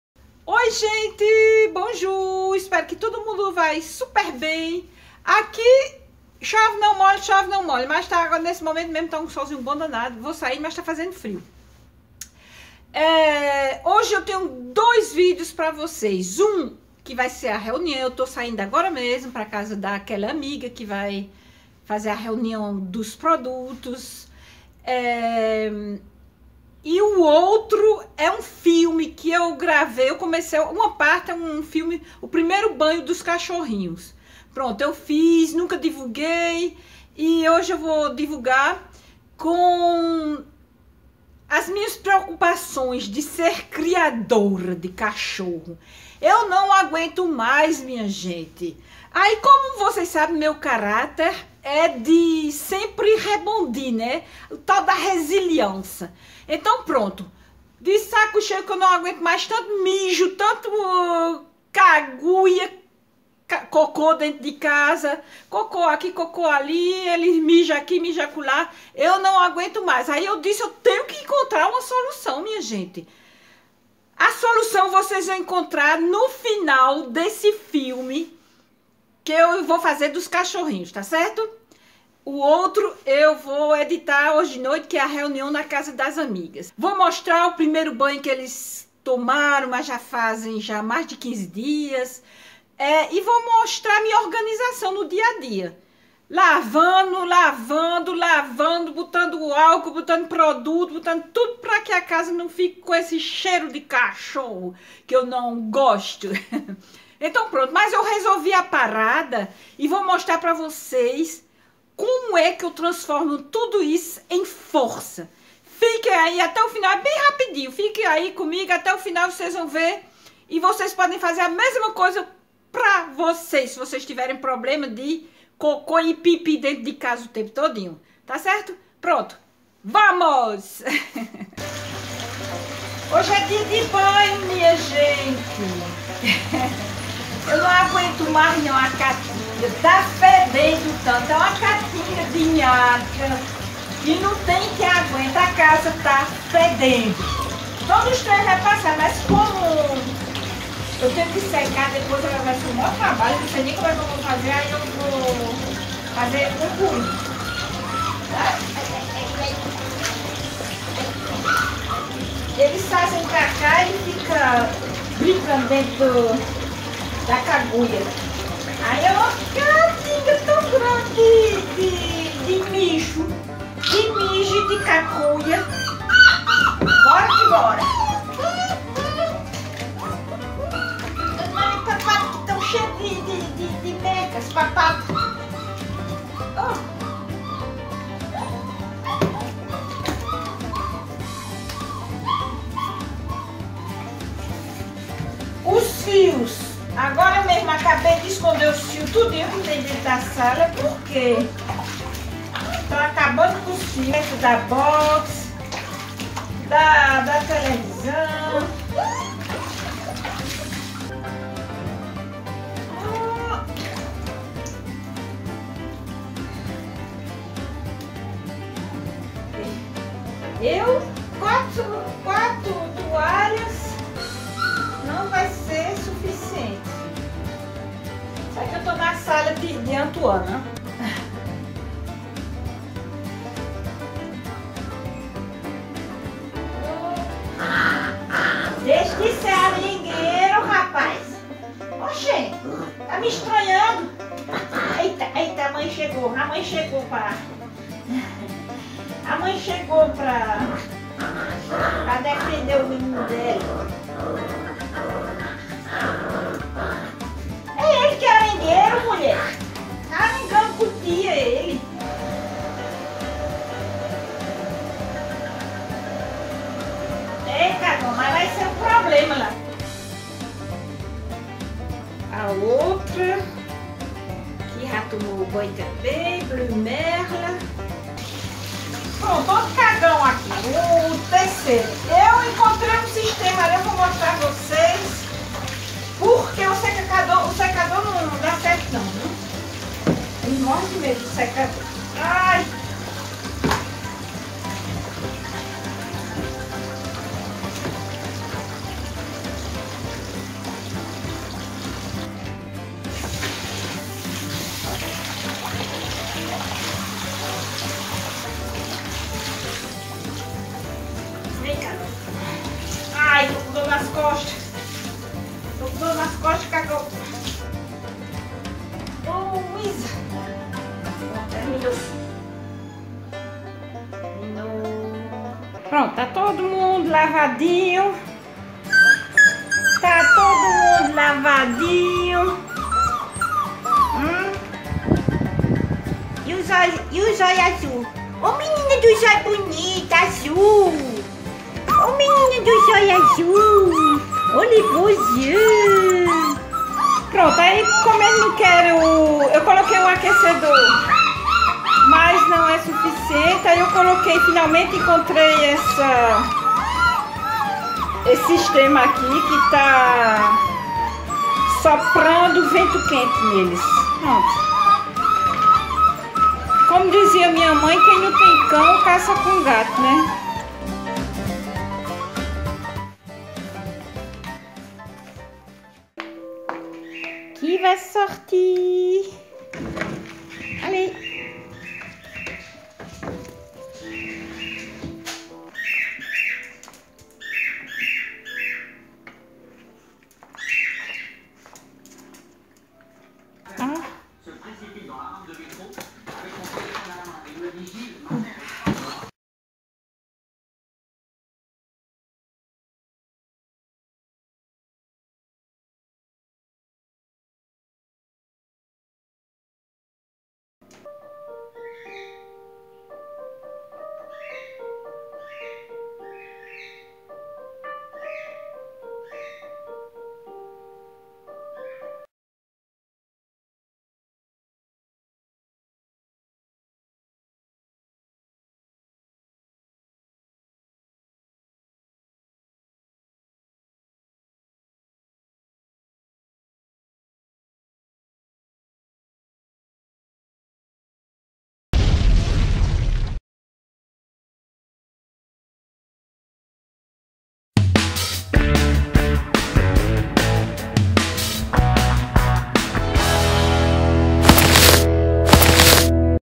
CACHORRINHOS BONITINHOS MAS......
Primeiro banho dessas pestinhas fedorentas, images roubadas de Antoine meu filho que também toca piano e eu transformando coco em €uros.